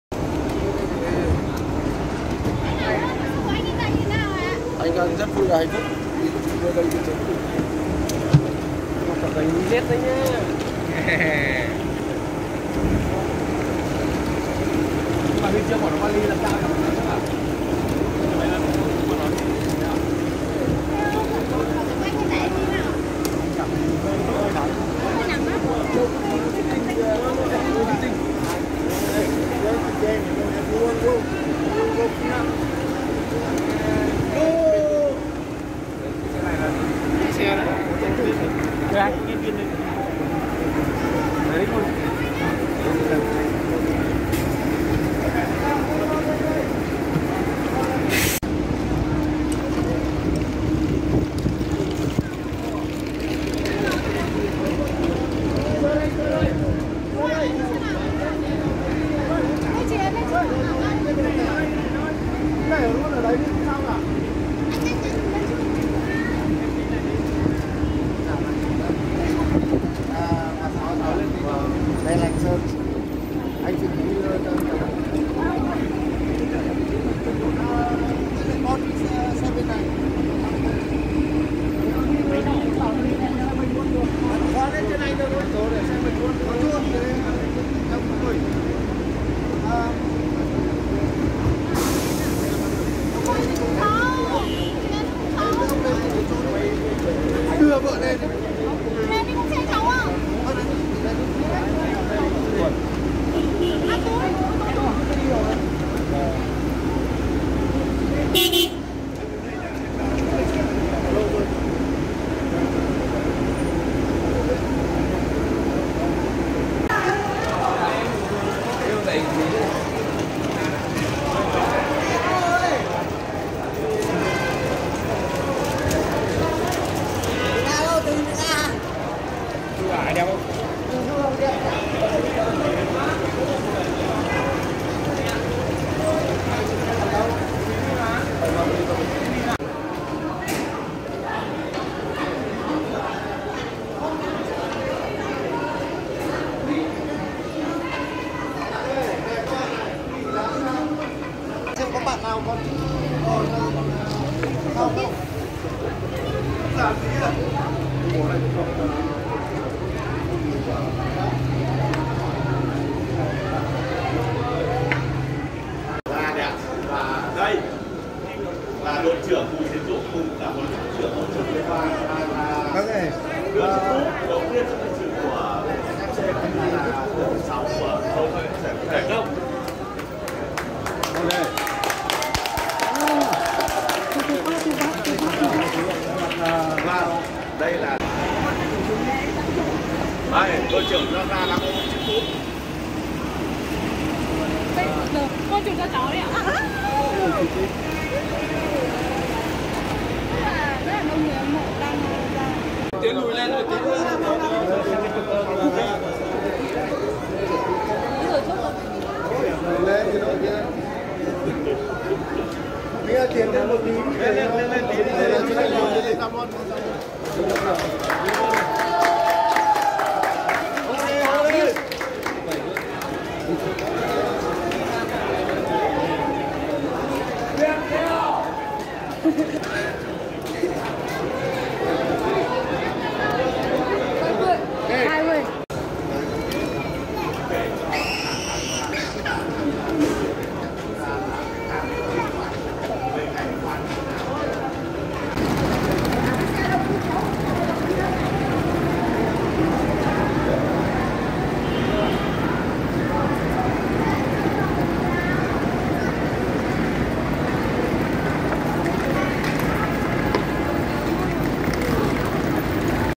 Viettel mang cúp vô địch V.League trở về báo công ở Hà Nội
Các cầu thủ Viettel đã mang cúp vô địch V.League trở về Hà Nội để báo công, trong sự chào đón của người hâm mộ.